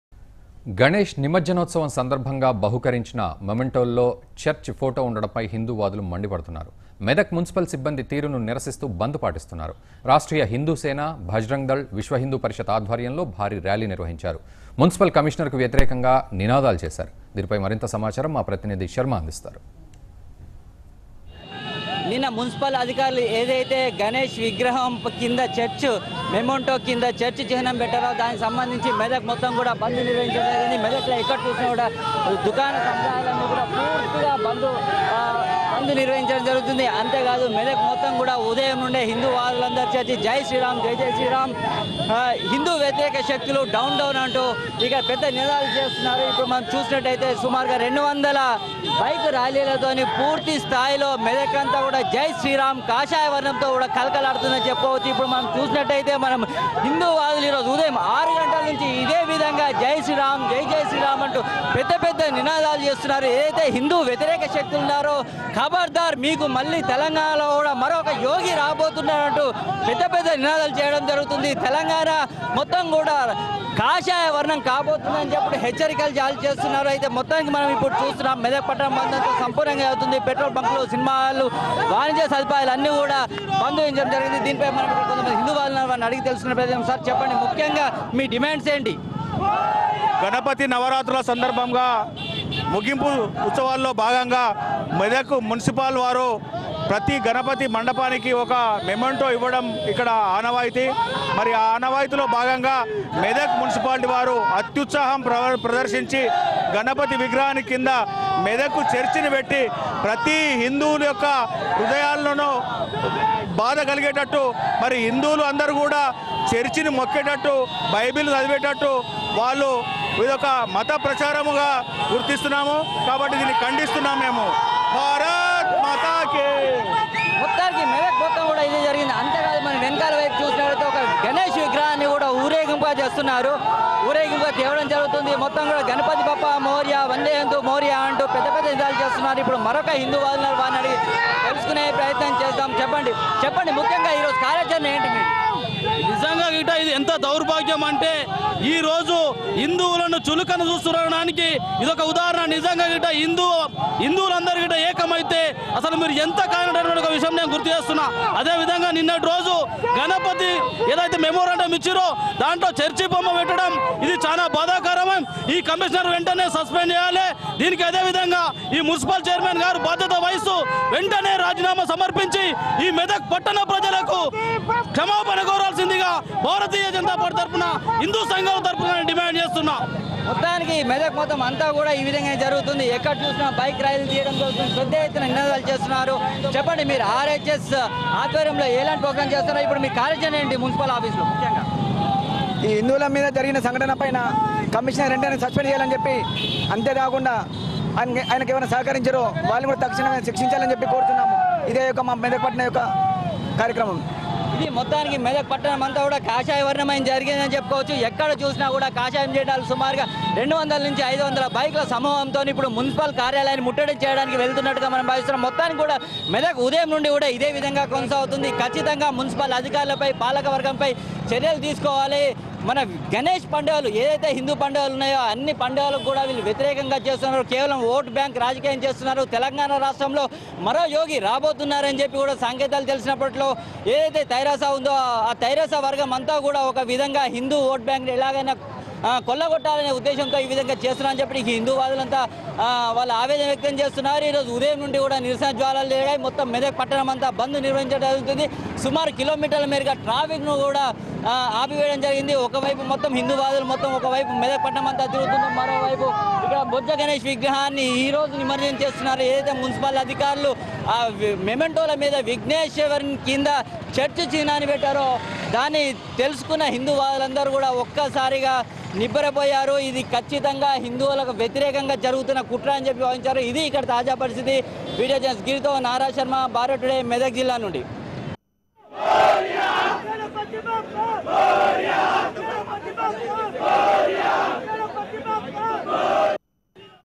Medak Bandh: VHP,RSS and Bajrang Dal Huge Rally Against Church Symbol In Ganesh Immersion
Medak Bandh: VHP,RSS and Bajrang Dal Huge Rally Against Church Symbol In Ganesh Immersion Momento | Bharat Today